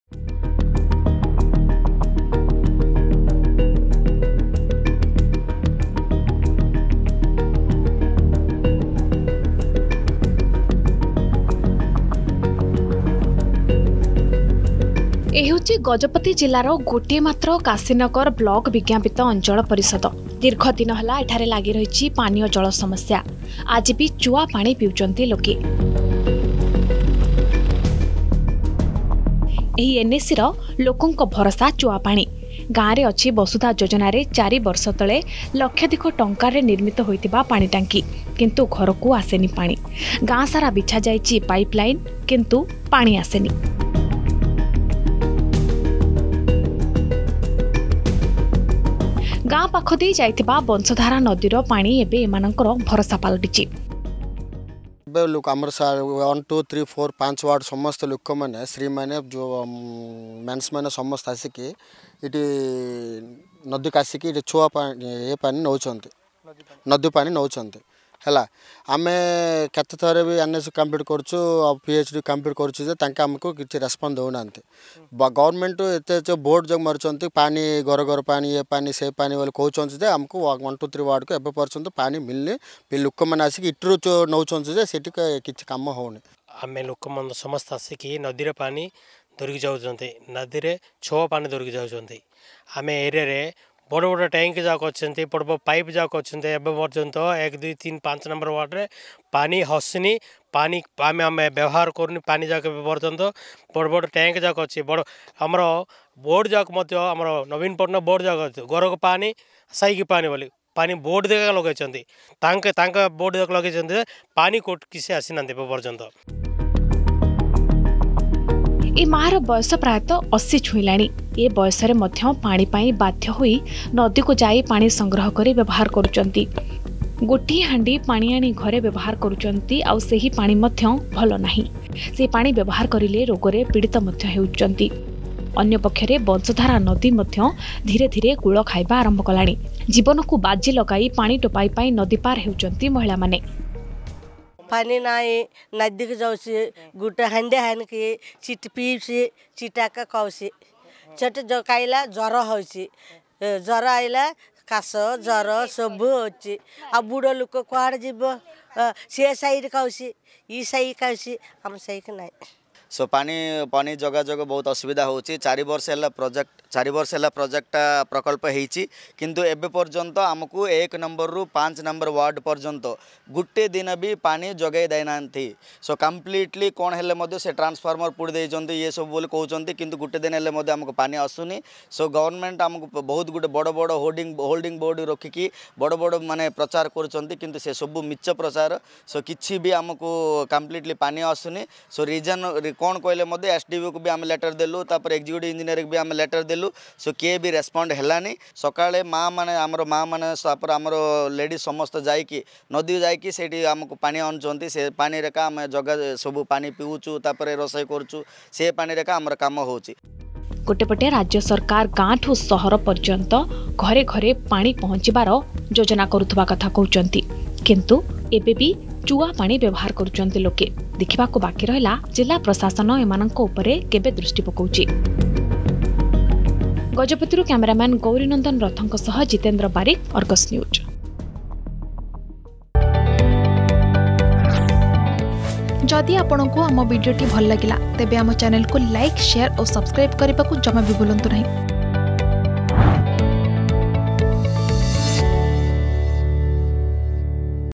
ଗଜପତି ଜିଲ୍ଲାରେ ଲୋକଙ୍କୁ ଭରସା ଚୁଆ ପାଣି || Gajapati water problem
ଗଜପତି ଜିଲ୍ଲାରେ ଗୋଟିଏ ମାତ୍ର କାଶୀନଗର ବ୍ଳକ ବିଜ୍ଞାପିତ ଅଞ୍ଚଳ ପରିଷଦ ଦୀର୍ଘ ଦିନ ହେଲା ଏଠାରେ ଲାଗି ରହିଛି ପାନୀୟ ଜଳ ସମସ୍ୟା ଆଜି ବି ଚୁଆ ପାଣି ପିଉଛନ୍ତି ଲୋକେ । ଏହି ଗାଁ ଲୋକଙ୍କୁ ଭରସା ଚୁଆ ପାଣି ।